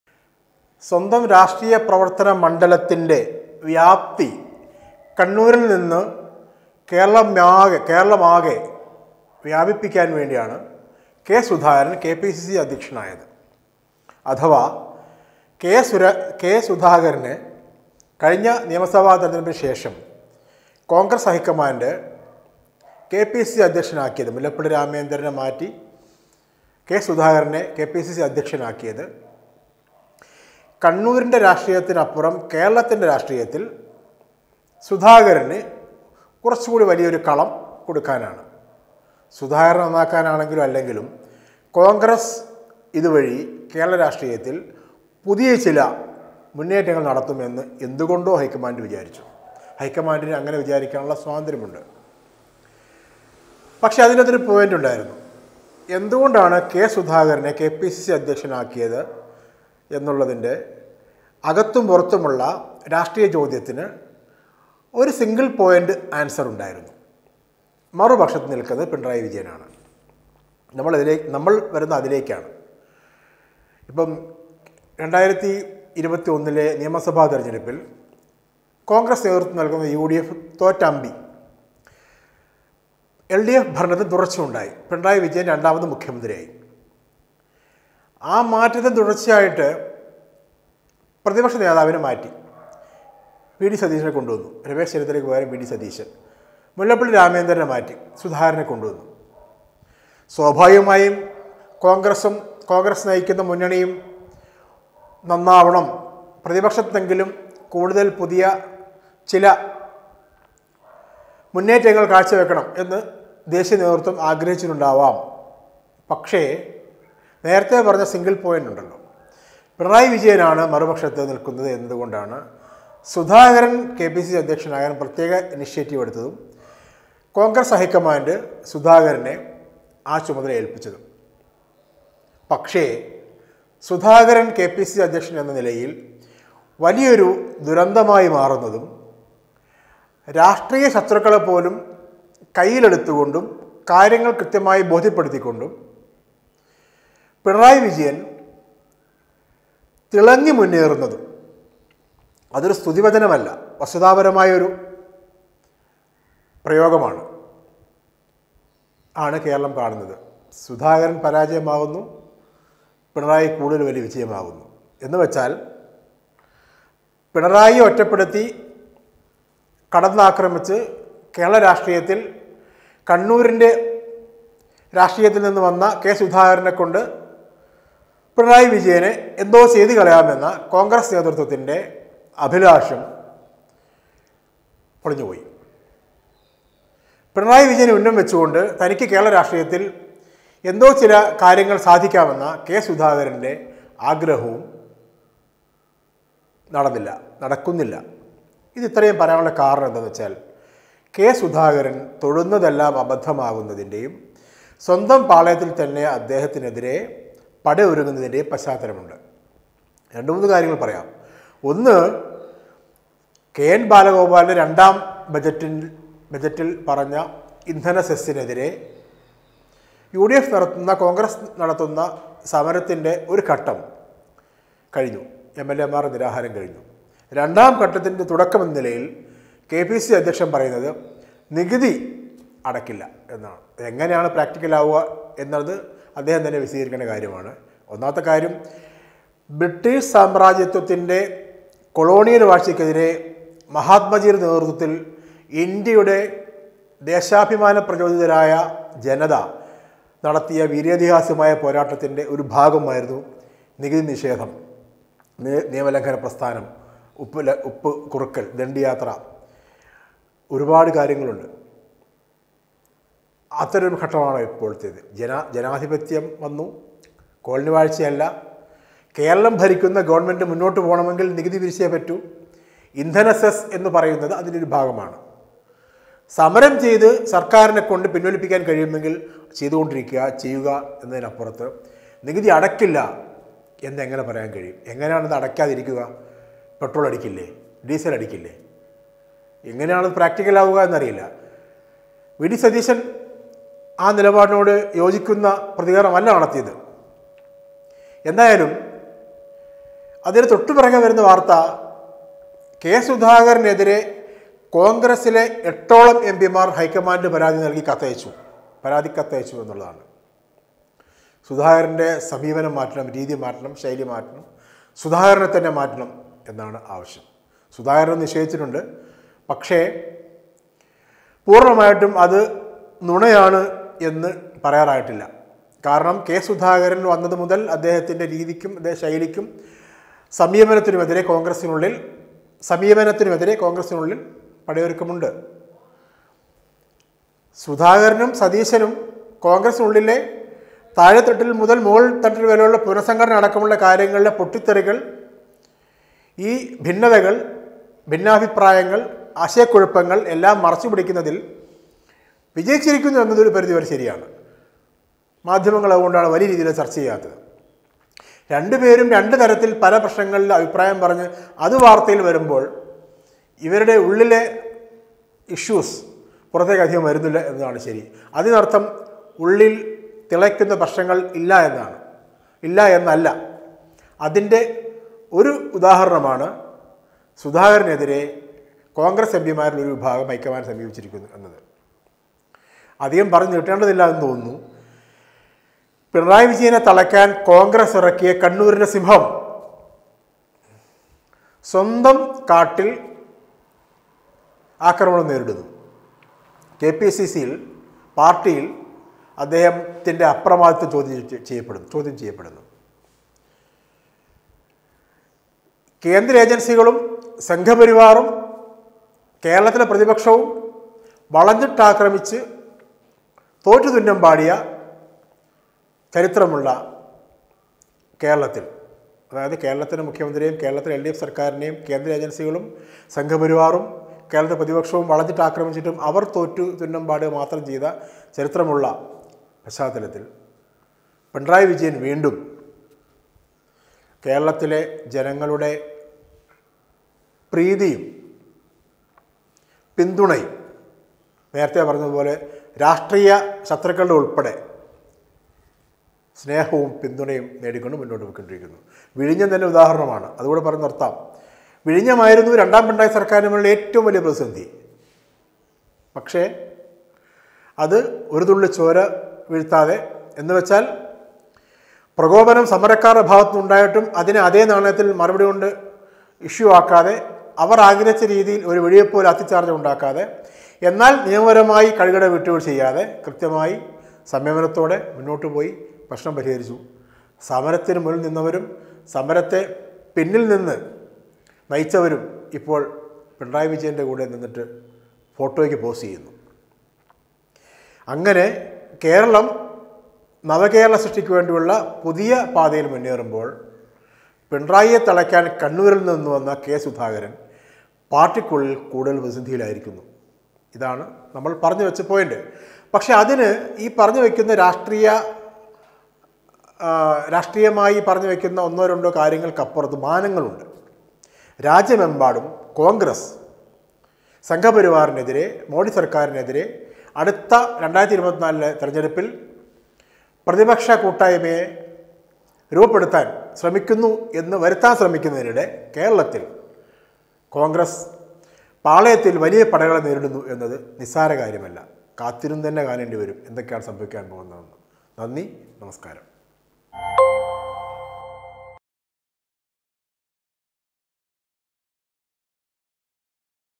പിണറായിയെ വീഴ്ത്താൻ കണ്ണൂരിൽ നിന്ന് സിങ്കത്തെ ഇറക്കി | സുധാകരന്റെ പതനം : പി എസ് റംഷാദ് പറയുന്നു
സിങ്കം മൂക്കുകുത്തി  വീണിട്ടും പിണറായി ഇപ്പോഴും അജയ്യൻ